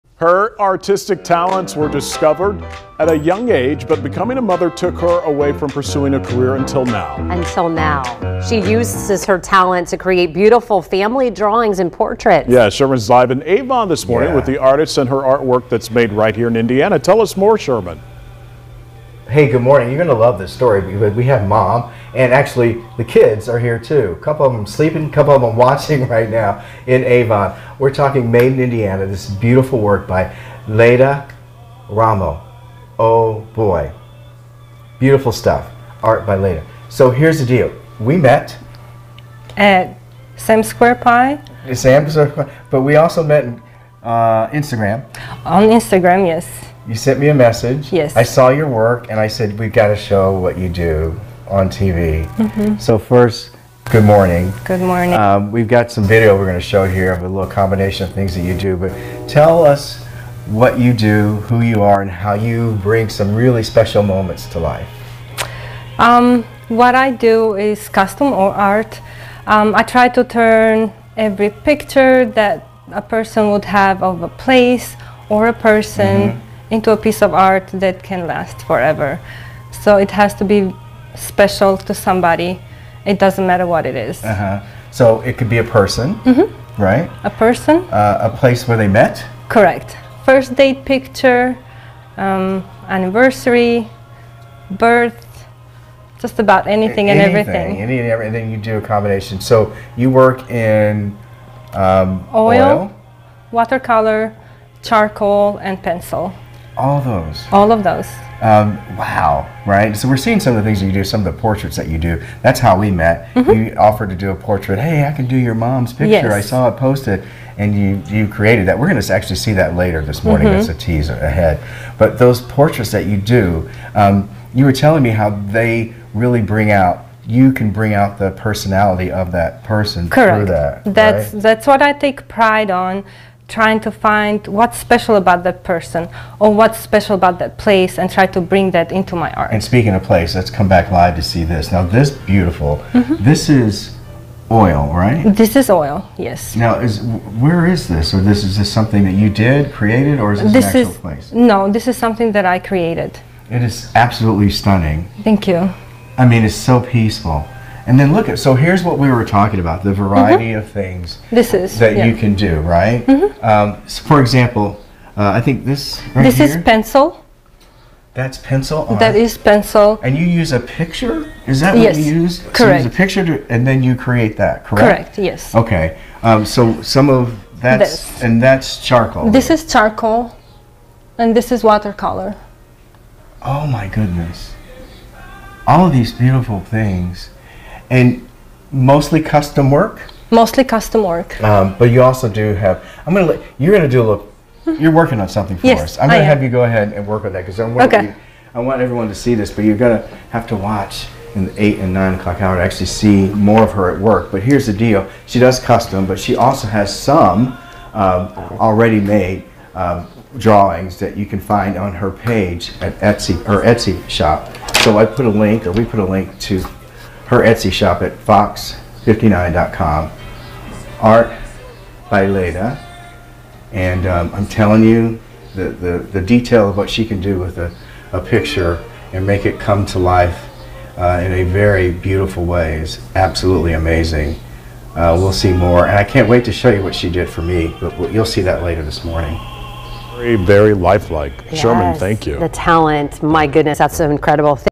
Where is Sherman? Art by Lejda
Her artistic talents were discovered at a young age, but becoming a mother took her away from pursuing a career until now. Now she uses her talent to create beautiful family drawings and portraits. Sherman went to Avon to introduce us to the artist.